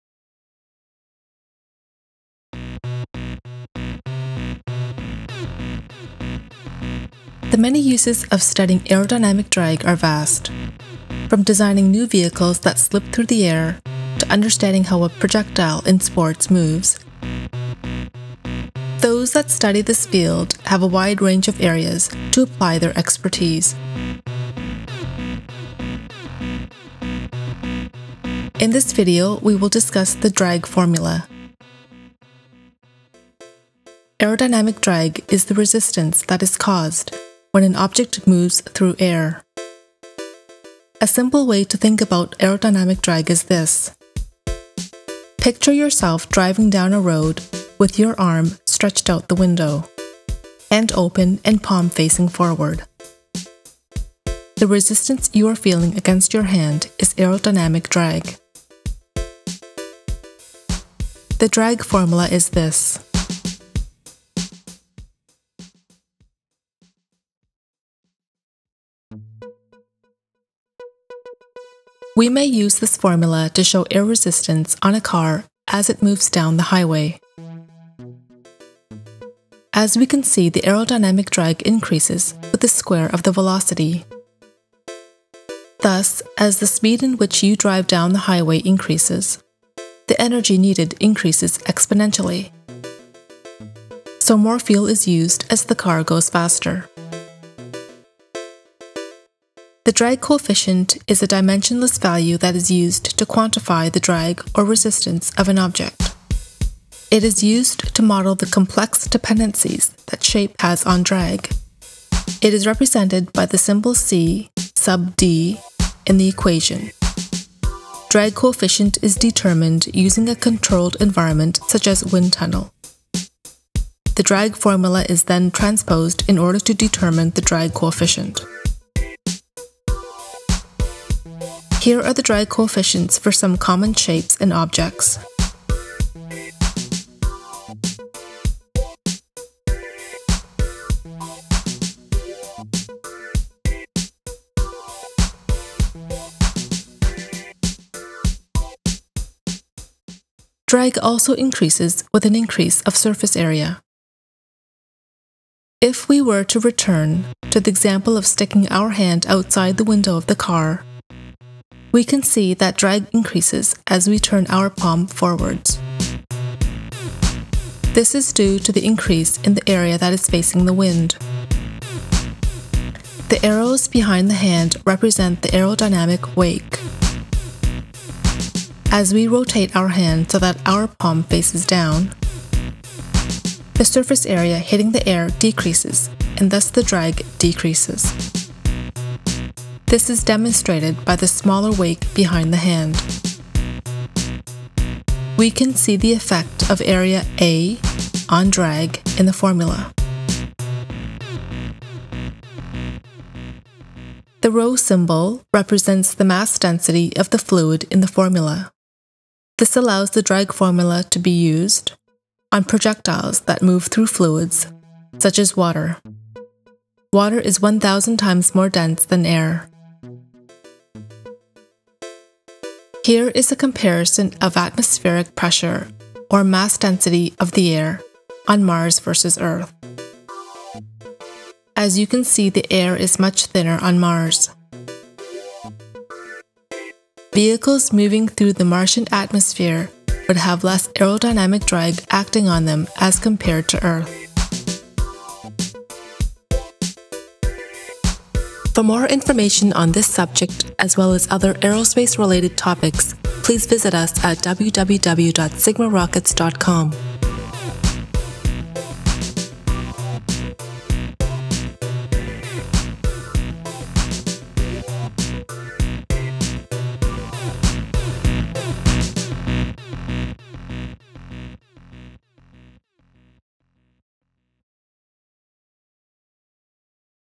Understanding Aerodynamics - The Drag Formula
In this video we discuss the Drag Formula.  We break down each variable in the Drag Formula and show examples of how the formula is affected by each one.  This video is part of our lesson on aerodynamics